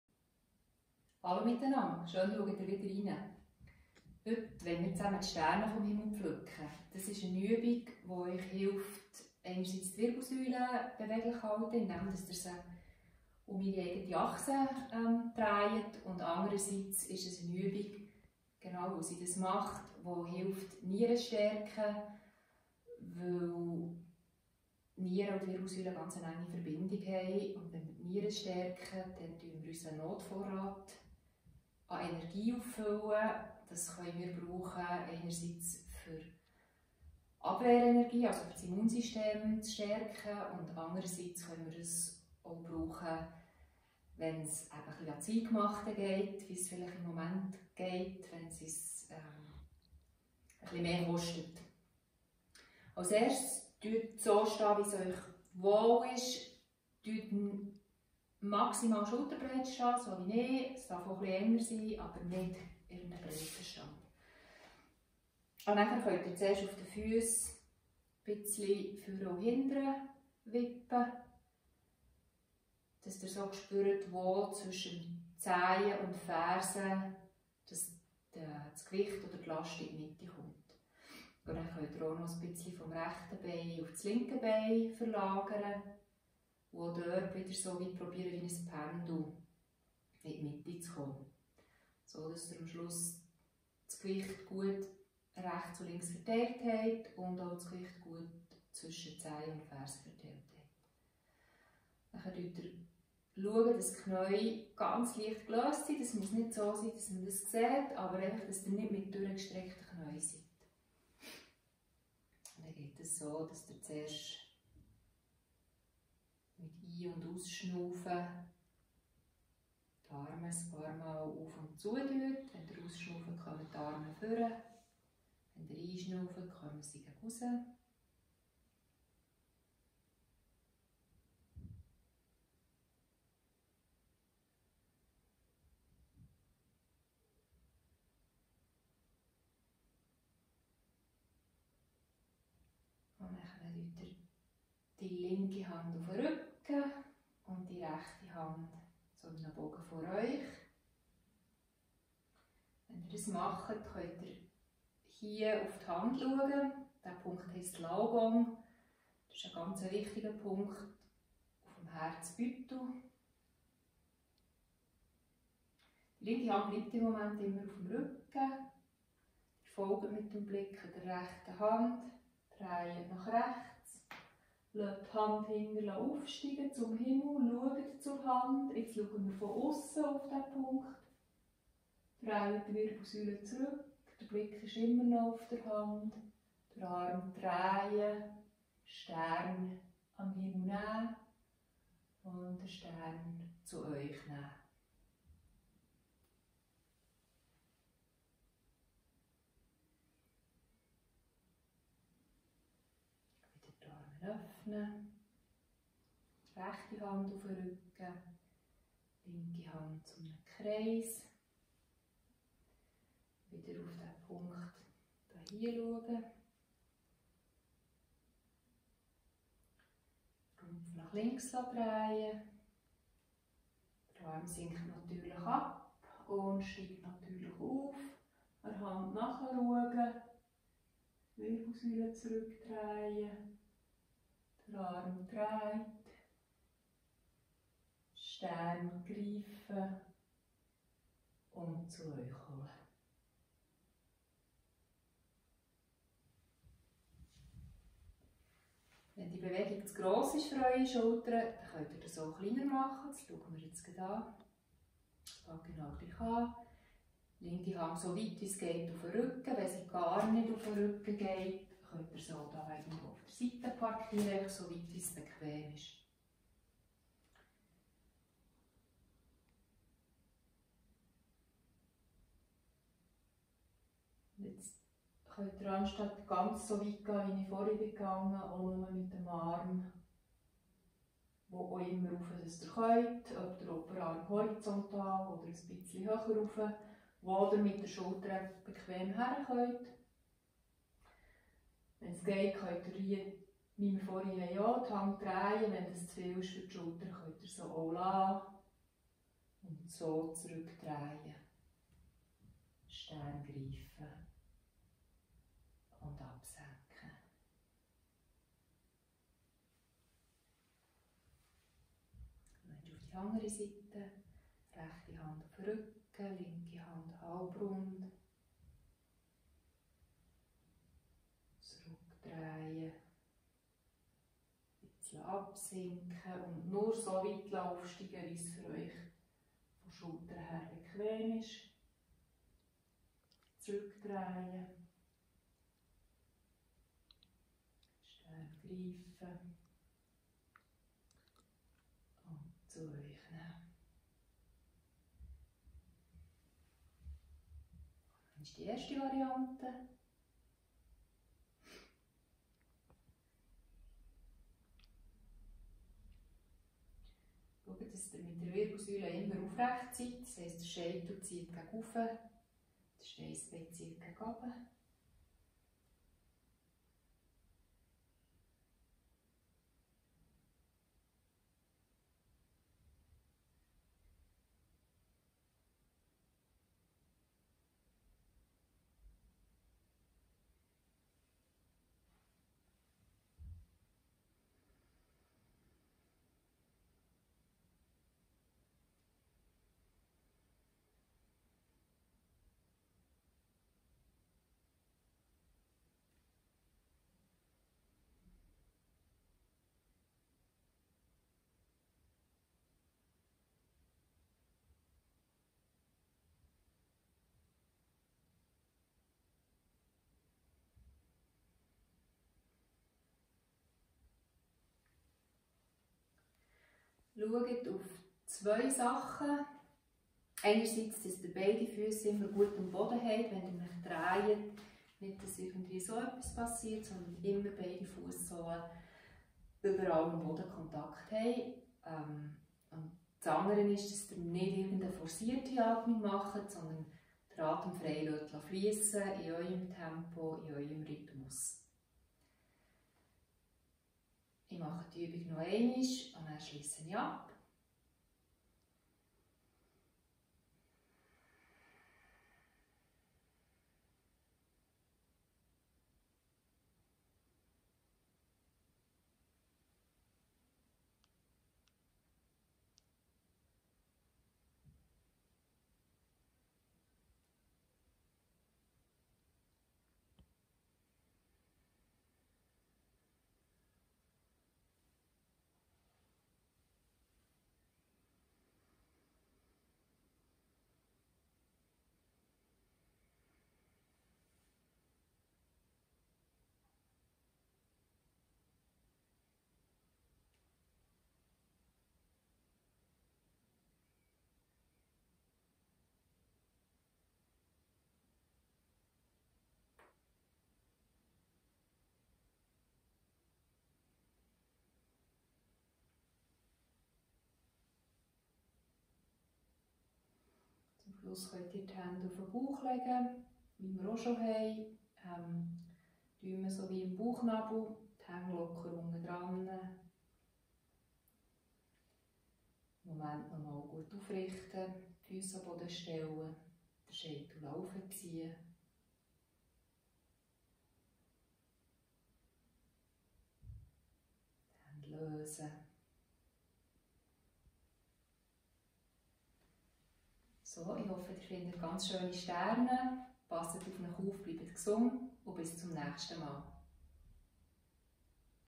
Die Sterne vom Himmel pflücken
Qigong Übung zum Beleben der Wirbelsäule und der Nieren